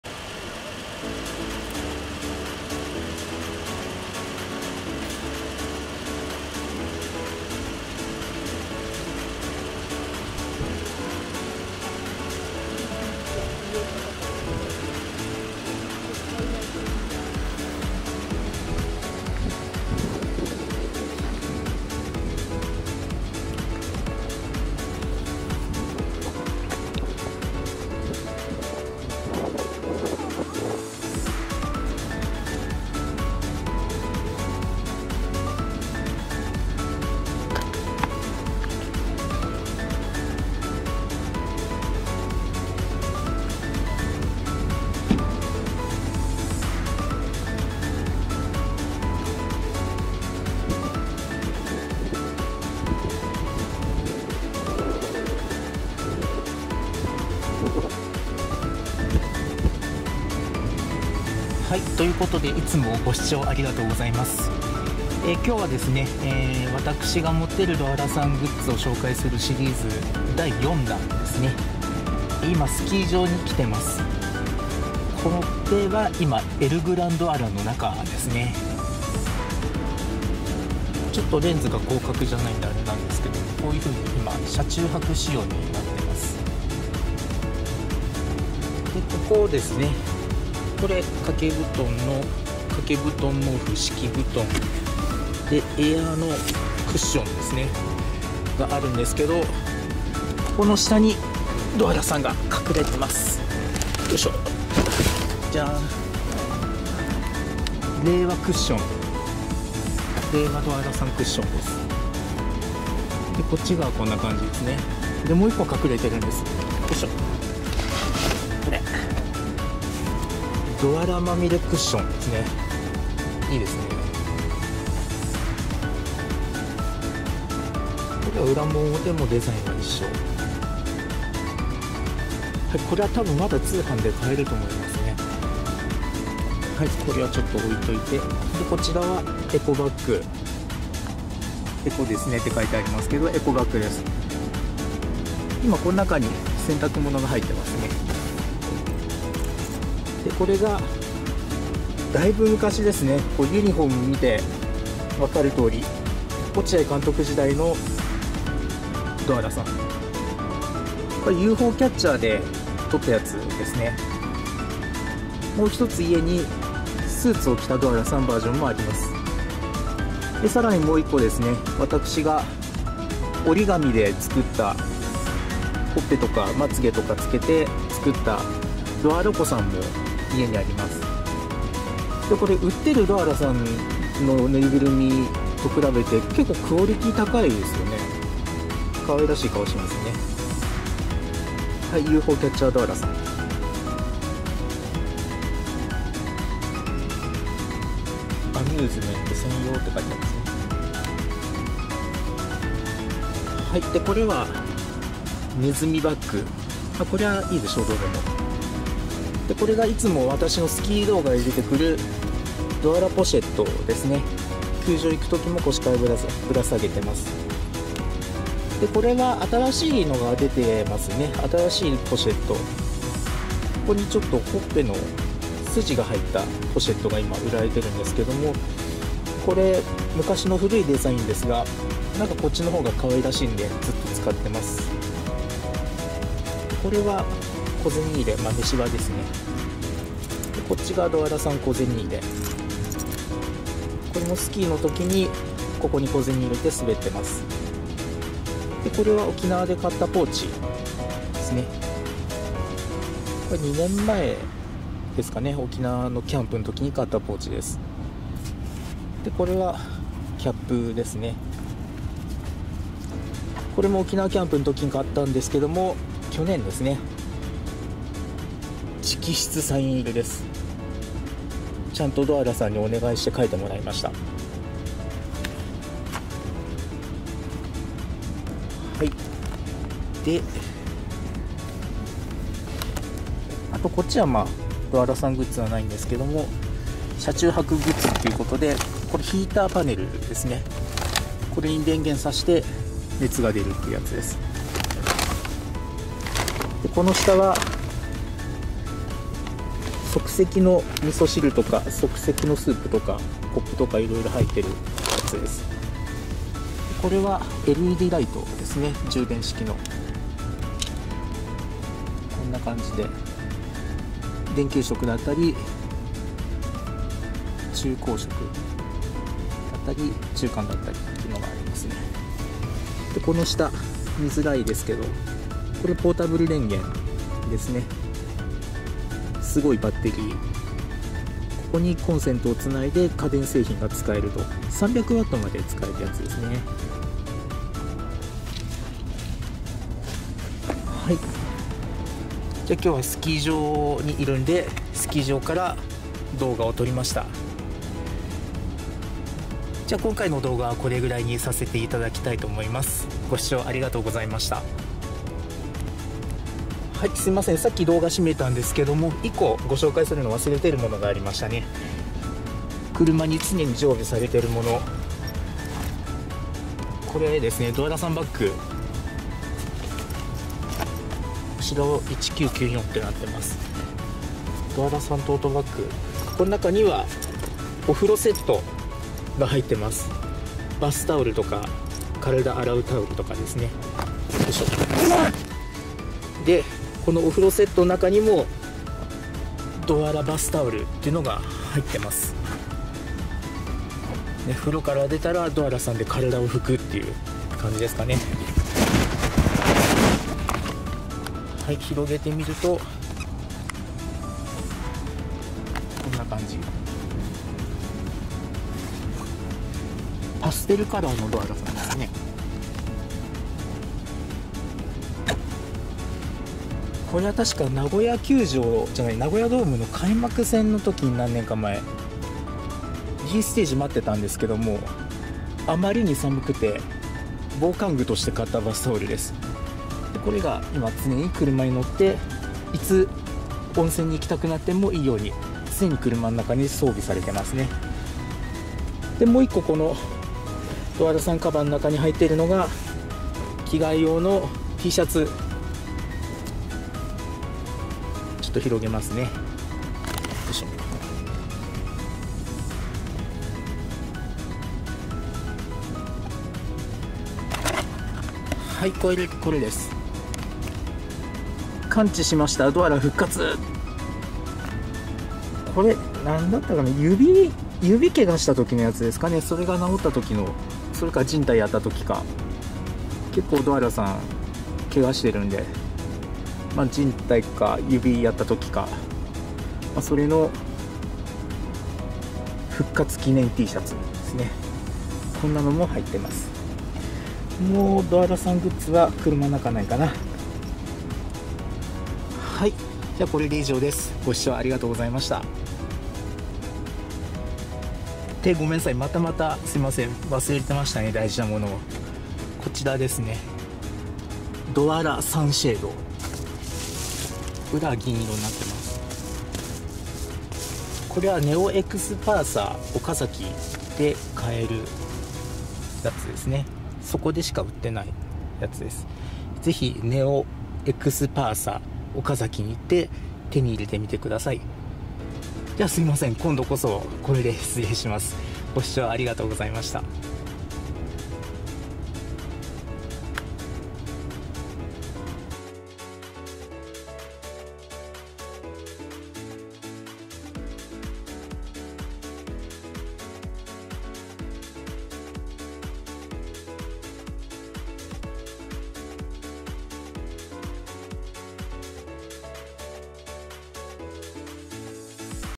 うｐ主が持ってるドアラグッズシリーズ その4「エルグランドアラ 350Highway STAR 4WD Ski Ver.」
ドアラステッカー、ドアラぬいぐるみホーム＆ビジター、ドアラポシェット、ドアラ令和クッション、ドアラまみれクッション、ドアラ小銭入れ、ドアラエコバッグ、ドアラトート、ドアラバスタオル、ドアラサンバイザー等盛りだくさん！！